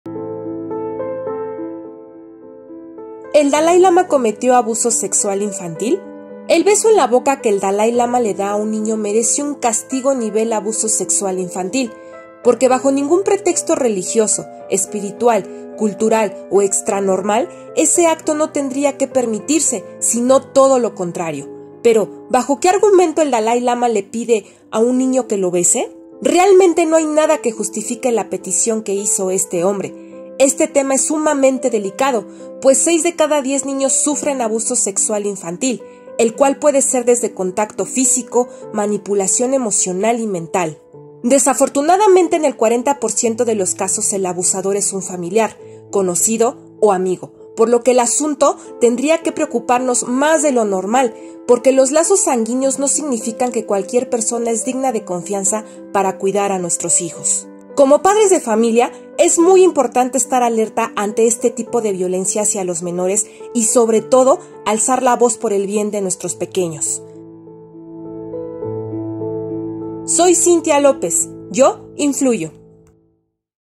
¿El Dalái Lama cometió abuso sexual?
El Dalái Lama, una de las principales figuras del budismo, se ha visto envuelto en varias polémicas públicas por declaraciones o comentarios considerados impropios.